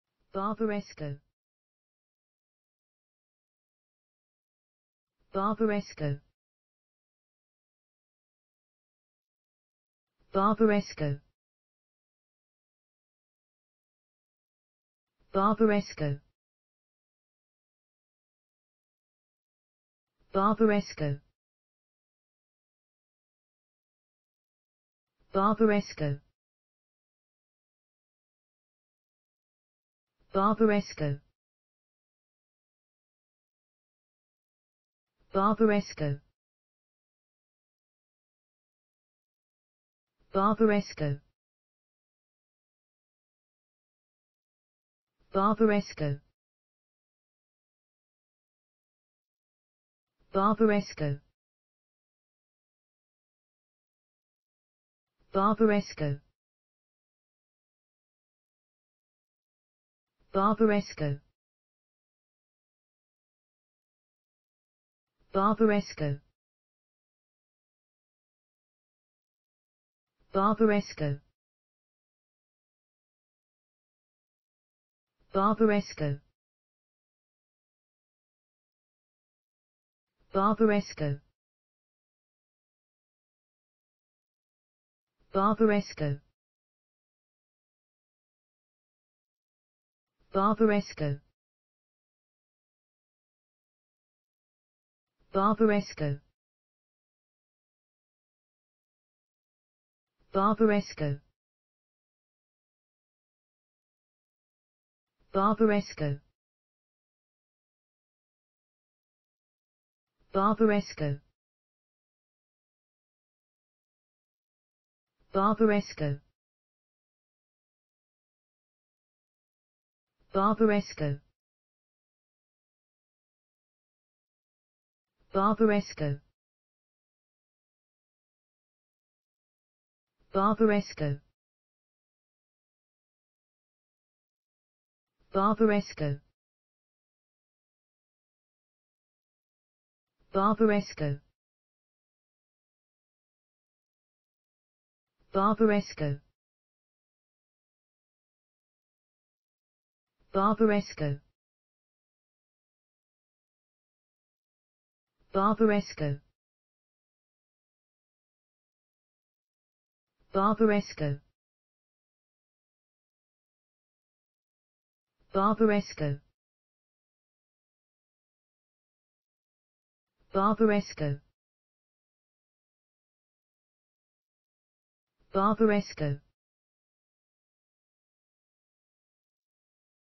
Barbaresco: How to pronounce Barbaresco in english (correct!).Start with B. Learn from me.
Follow me!Learn from me how to pronounce the word Barbaresco in english.This video shows you how to pronounce the word Barbaresco in english.

Barbaresco

Barbaresco is an Italian wine made with the Nebbiolo grape. Barbaresco is produced in the Piedmont region in an area of the Langhe immediately to the east of Alba and specifically in the comunes of Barbaresco, Treiso and Neive plus that area of the frazione San Rocco Senodelvio which was once part of the comune of Barbaresco and now belongs to the comune of Alba.